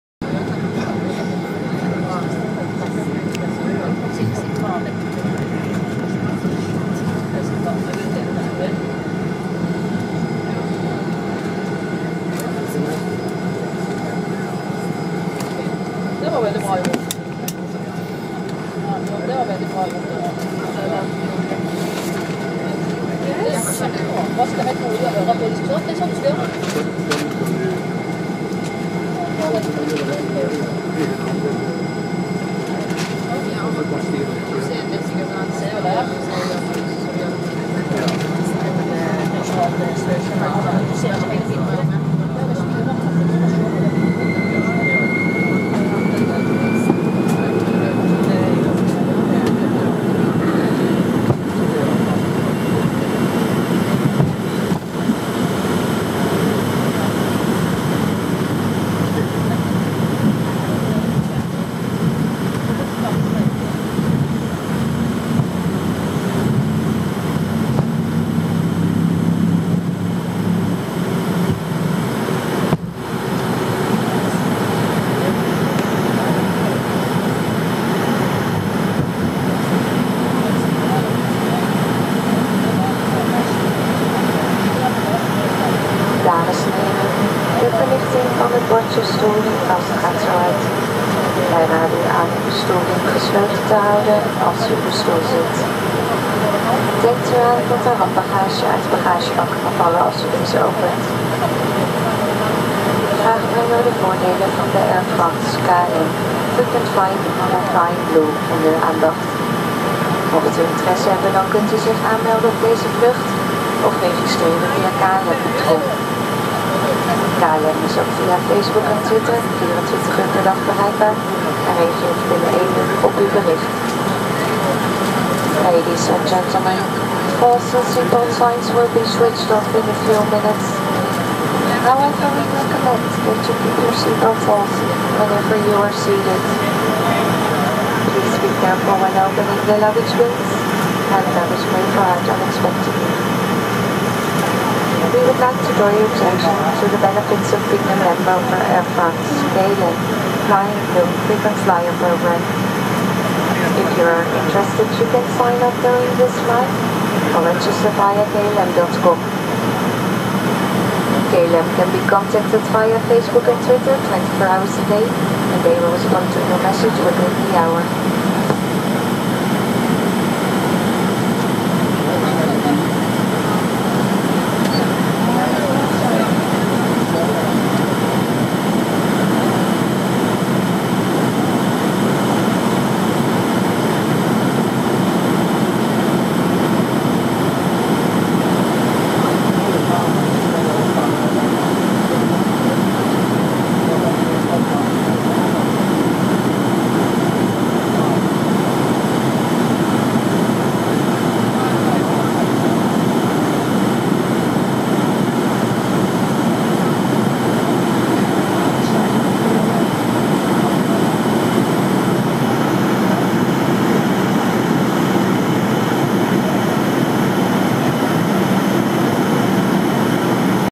Stavanger, Taxi andTake off in Stavanger, KL 1200, SVG AMS, Runway 29, Fokker 70, PH-KZI
Stavanger, Taxi andTake off in Stavanger,
Stavanger (SVG)- Amsterdam (AMS),  KL 1200,
Runway 29, Fokker 70, PH-KZI
2015|09|09